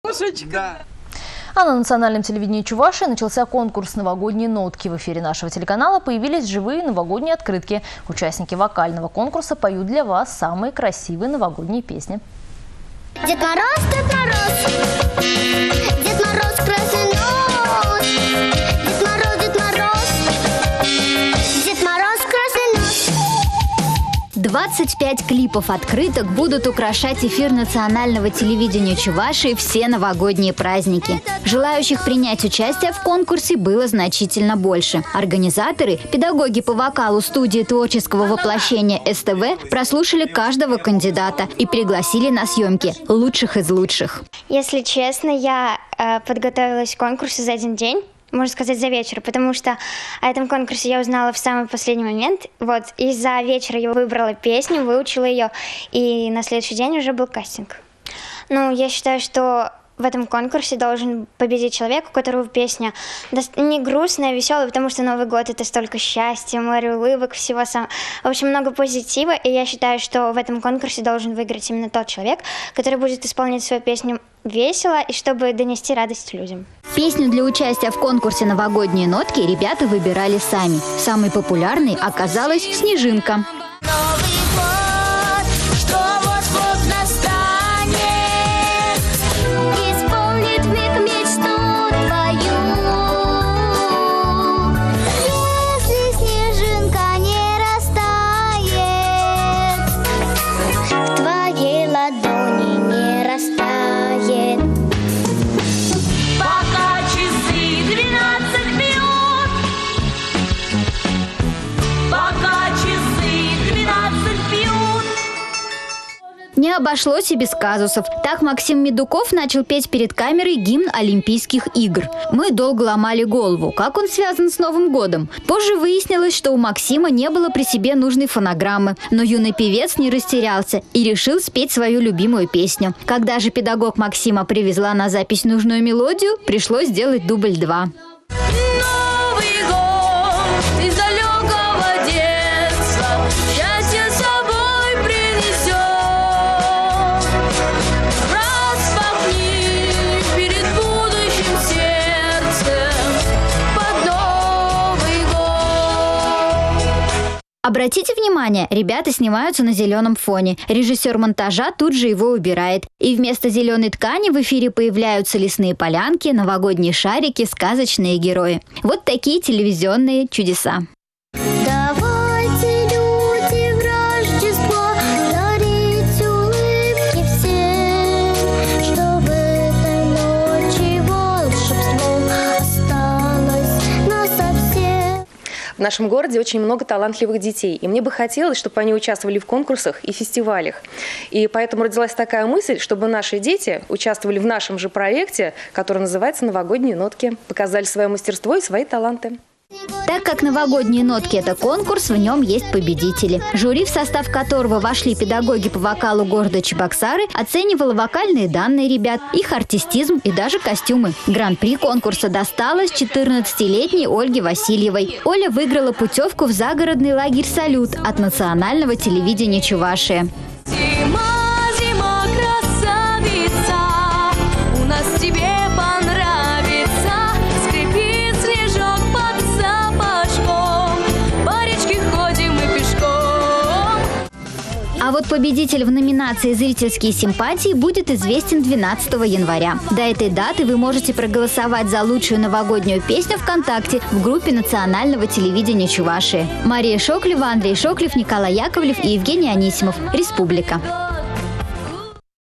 На Национальном телевидении Чувашии начался конкурс «Новогодние нотки»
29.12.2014
В эфире нашего телеканала появились  живые новогодние открытки.   Участники  вокального конкурса поют для вас самые красивые новогодние песни.